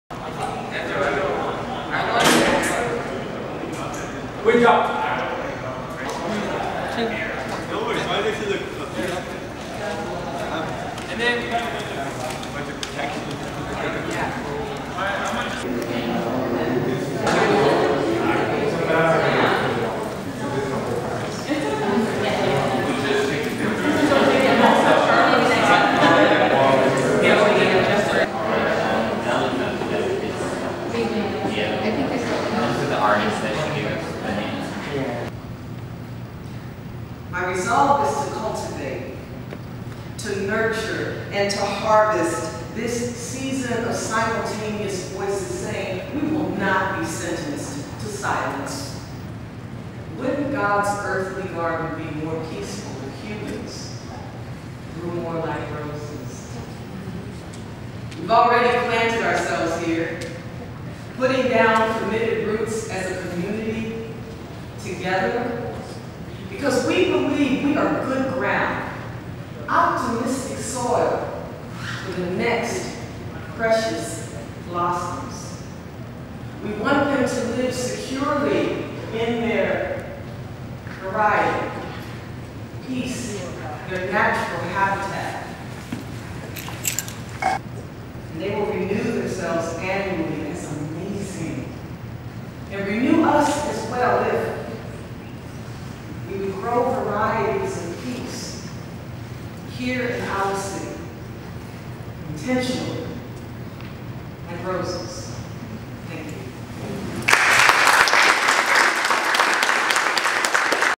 Carlos Javier Ortiz Art exhibition opens at L.H. Horton gallery
Delta College's L.H. Horton Art gallery Show Cases photographs by Carlos Javier Ortiz as well as spoken word by Tama Brisbane. VIDEO BY ALEX COBA.